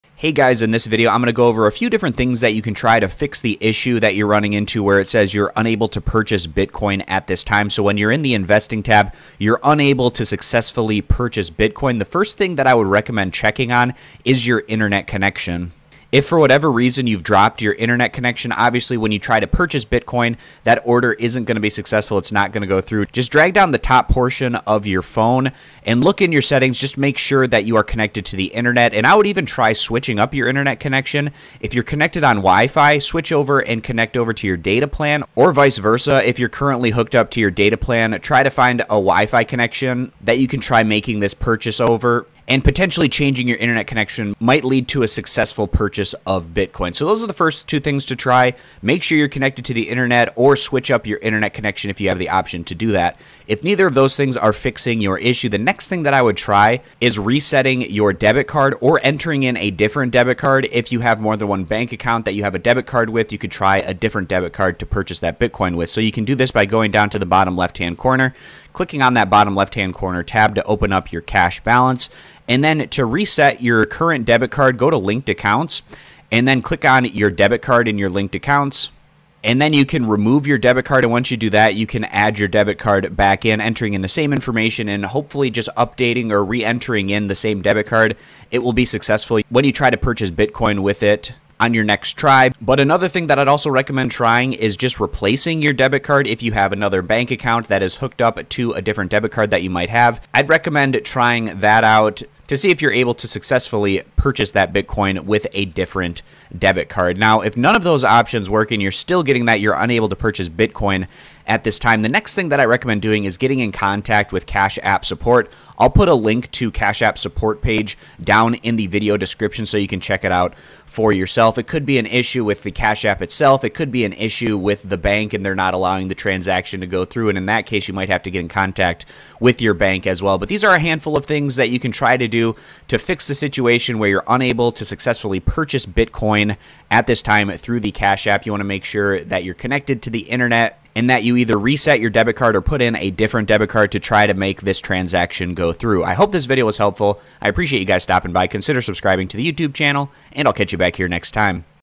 Cash App Unable To Purchase Bitcoin at This Time - Cash App Unable To Buy Bitcoin at This Time Help
Use My Referral Code at Signup For Bonus - Referral Code: WJQMHPZ

Hello, In This Video I Go Over Cash App Unable To Purchase Bitcoin at This Time and Cash App Unable To Buy Bitcoin at This Time. Unable To Purchase Bitcoin Cash App Step by Step Instructions, Guide, Tutorial, Video Help

In this video I walk through the step by step guide, instructions, tutorial on how to fix being unable to purchase buy Bitcoin through your Cash App. I Hope This Video Helped!